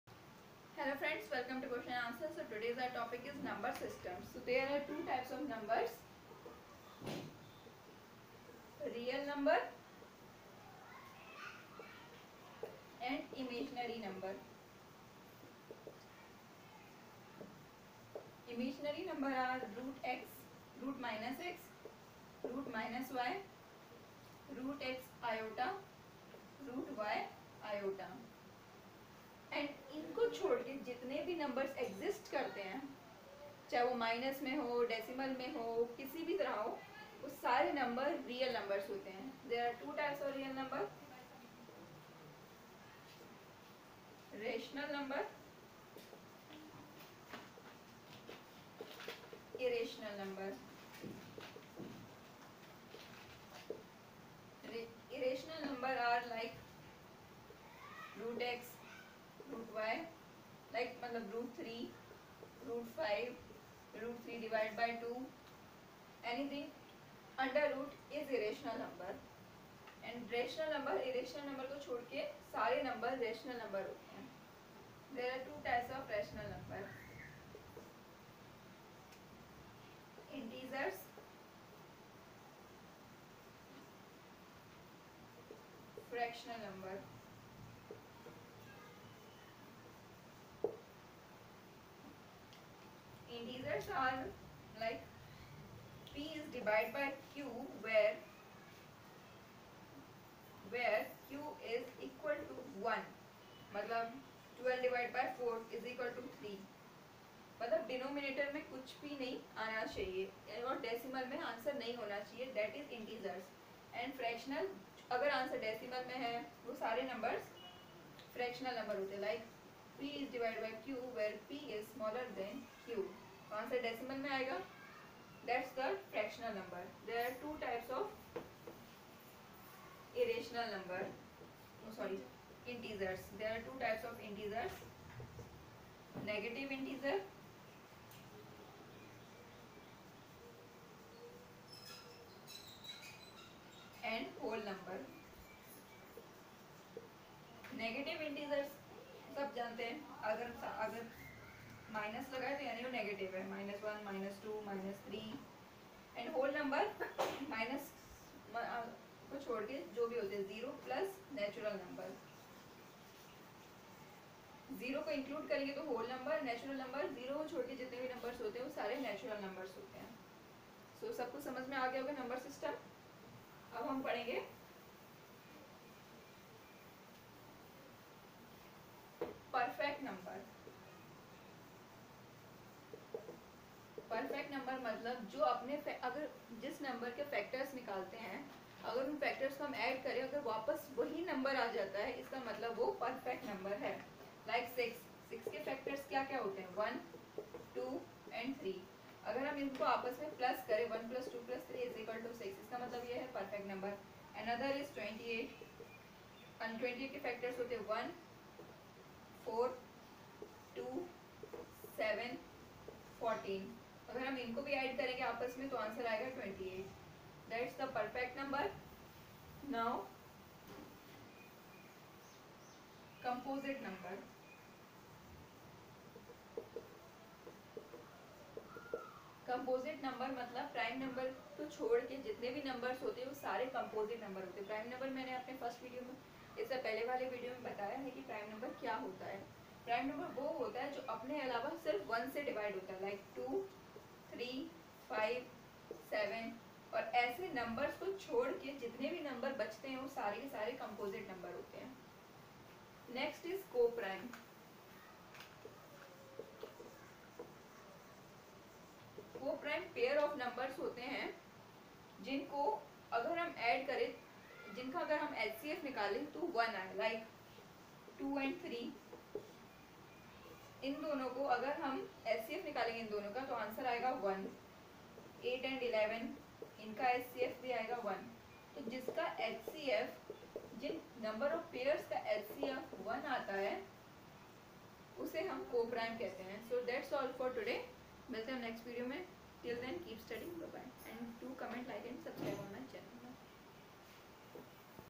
Number System: What is Number System ???? What are Perfect and Composite Number??? QUESTION N ANSWER
We discuss briefly about number system and perfect prime co-prime and composite numbers